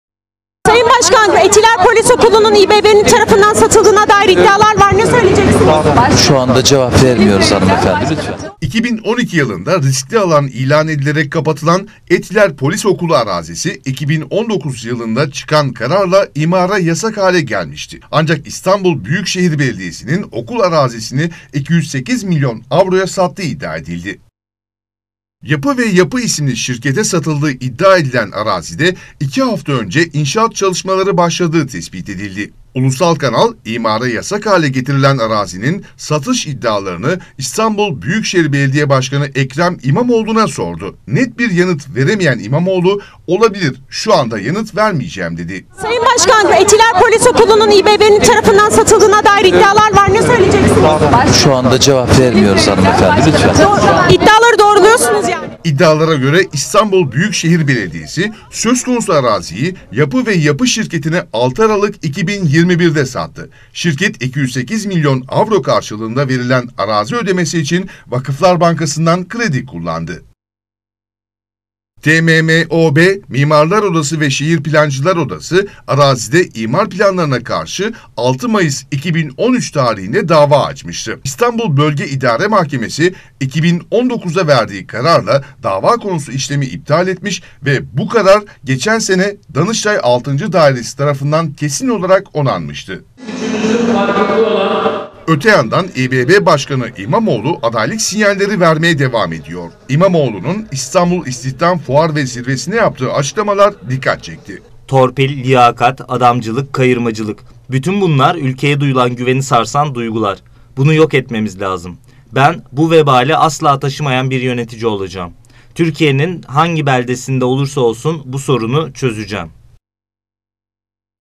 İBB Polis Okulu arazisini sattı mı?
Ulusal Kanal, imar yasağı olan “İstanbul Etiler Polis Okulu” arazisinin İstanbul Büyükşehir Belediyesi tarafından satıldığına ilişkin iddiaları, Ekrem İmamoğlu'na sordu. İBB Başkanı'nın yanıtı ise, "Olabilir, şu anda yanıt vermeyeceğim." oldu. İmamoğlu, daha sonra katıldığı etkinlikte adaylık sinyali verdi.